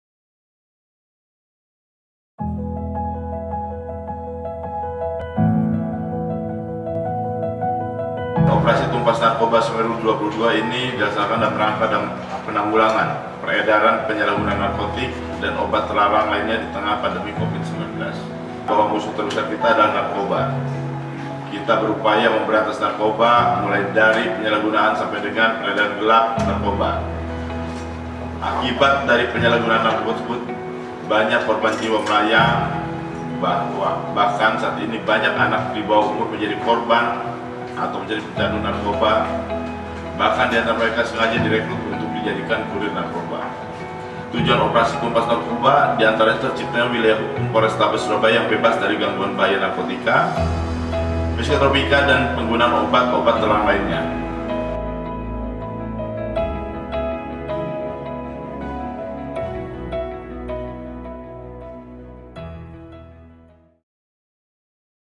LATPRAOPS TUMPAS NARKOBA 2022.
Kapolrestabes Surabaya, Kombes Pol Akhmad Yusep Gunawan pimpin Latpraops Tumpaas Narkoba di ruang M.Yasin, Sabtu (20/8/2022).

Giat operasi ini bertujuan untuk menciptakan kondusifitas di Kota Surabaya yang akan dilaksanakan selama 12 hari ke depan, terhitung mulai 22 Agustus - 2 September 2022 dalam rangka penanggulangan peredaran dan penyalahgunaan narkotika, psikotropika dan obat terlarang.